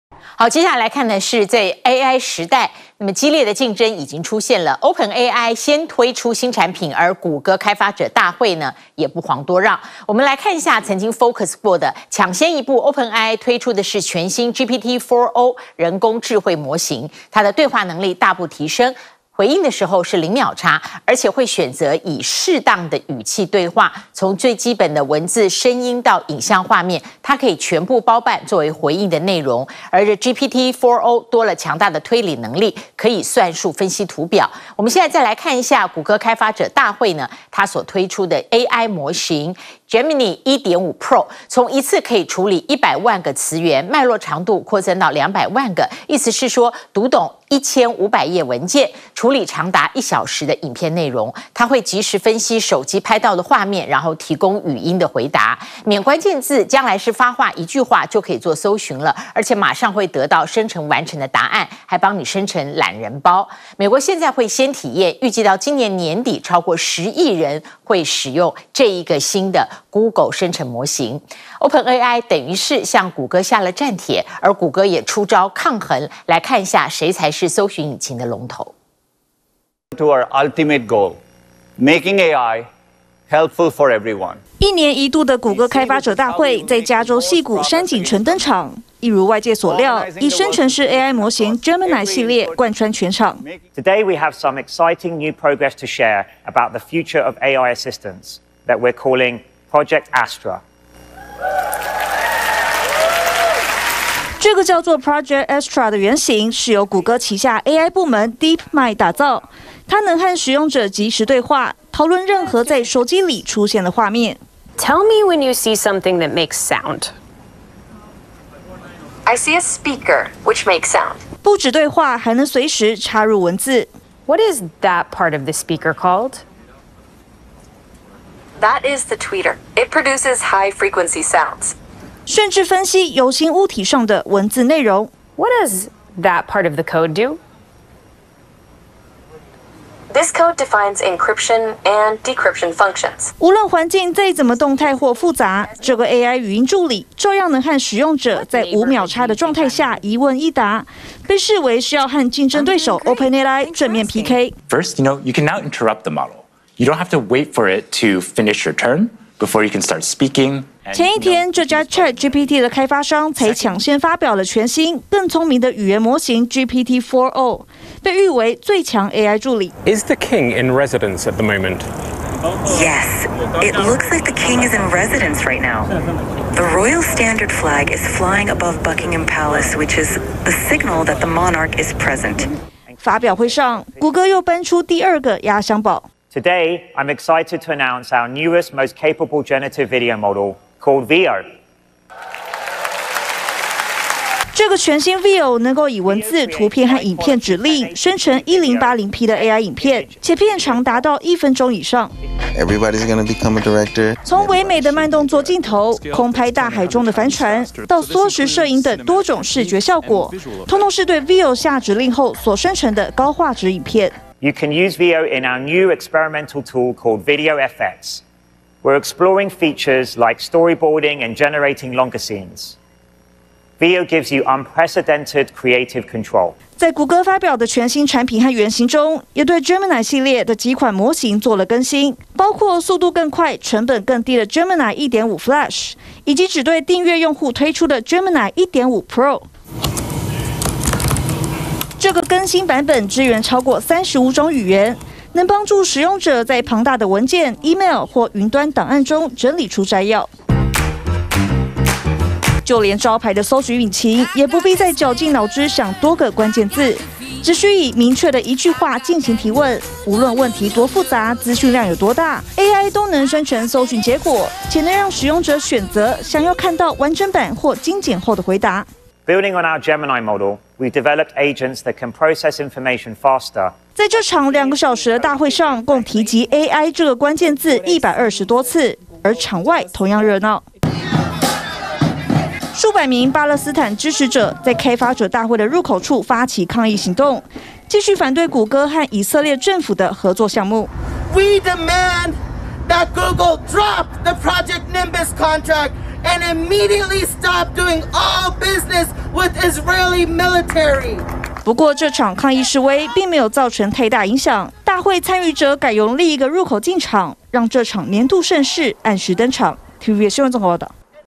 谷歌發表Gemini新模型! 生成AI影片 搜尋引擎.免想「關鍵字」【金臨天下XFOCUS】
📍熱門主題📍
99%美國人明年財務更糟 高利率帳戶卻不用 台美AI大漲救經濟 看波浪位階找趨勢族群｜金臨天下 完整版 20231208